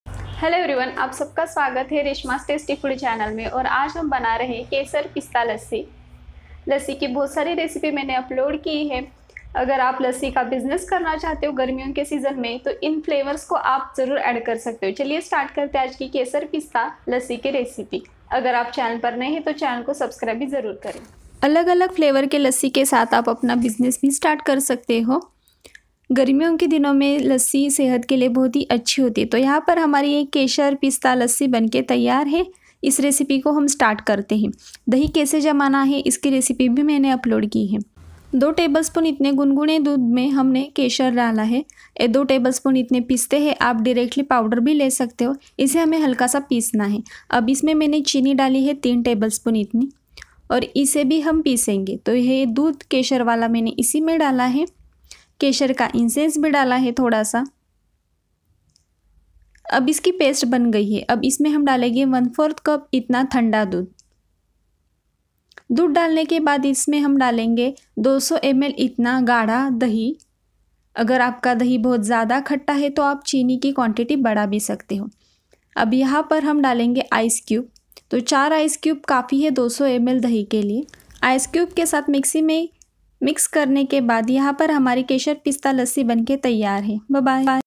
केशर पिस्ता लस्सी गर्मियों में दिलायेगा राहत keshar pista lassi recipe
**Ingredients :-
200 ml curd दही
3 tbsp powder sugar पिठीसाखर
4 icecubes बर्फ
keshar केशर

2 tbsp pistachio पिस्ता
1/4 cup chilled milk दुध

लस्सी कशी बनवायची
लस्सी कैसे बनाये
केशर लस्सी
पिस्ता लस्सी